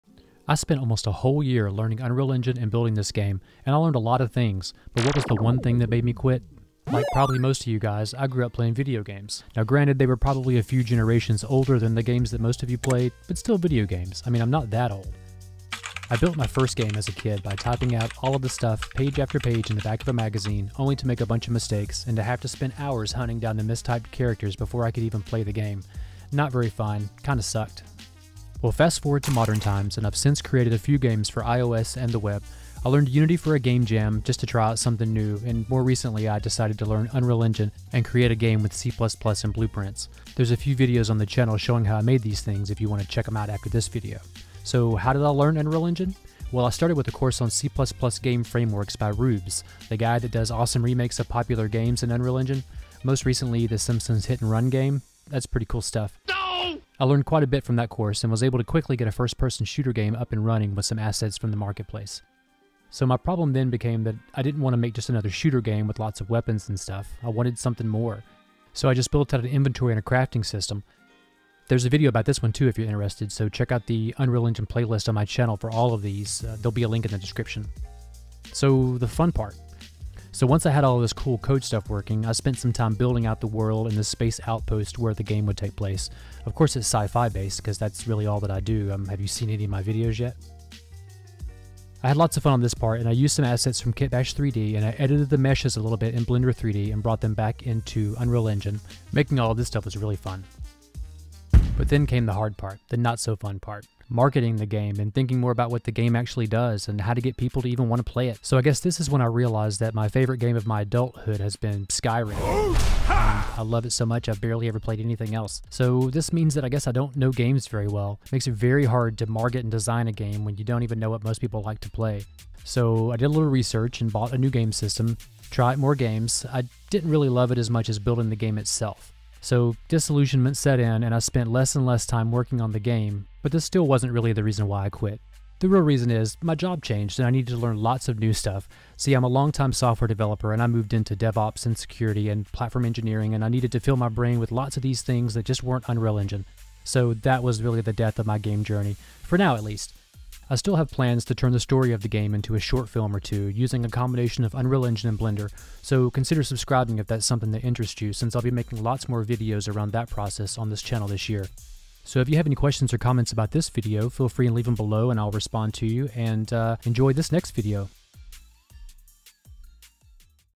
Why I Quit Game Dev (For Now?)
I guess I've officially quit game dev. This is the story of how I got here and what pushed me to this point.